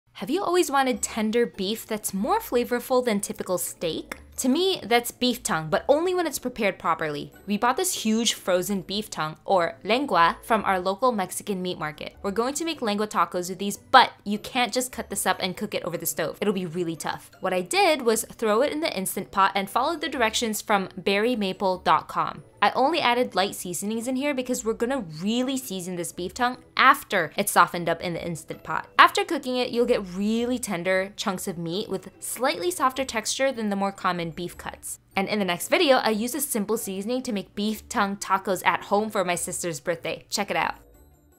Cooking beef tongue in an Instant Pot
Here was the recipe I used, but I also added chicken bouillon